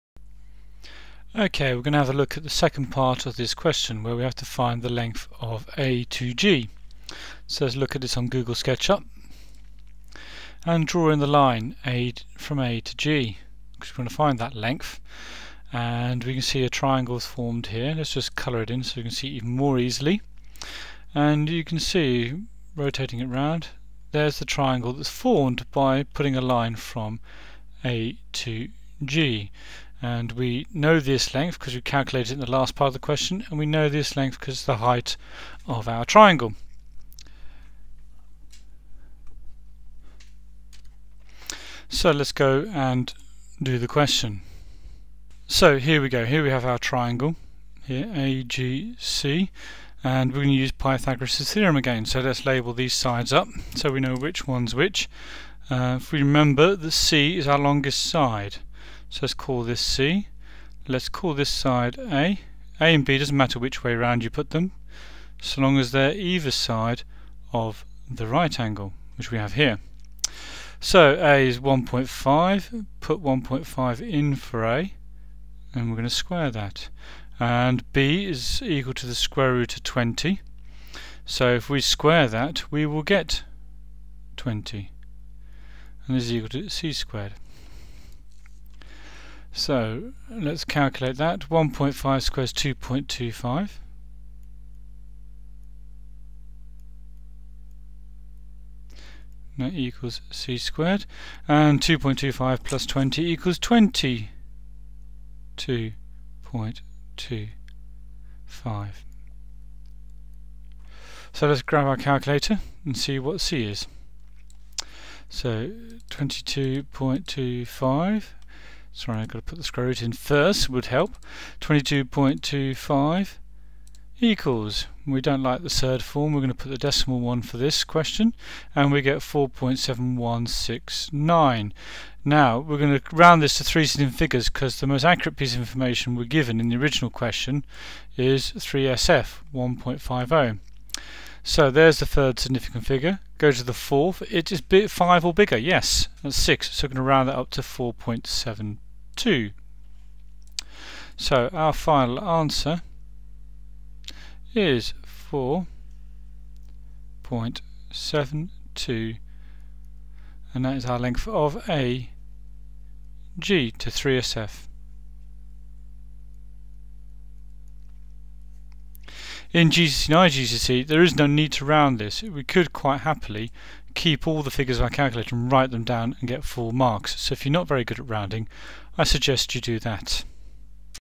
How to do Trigonometry in Three dimensions 3D Trig pythagoras cuboid part2.wmv
Part  2/5 of a 3D pythagoras/trig question. I will producing more videos. lessons will be free on youtube. You can order video answers to specific maths questions or exam papers.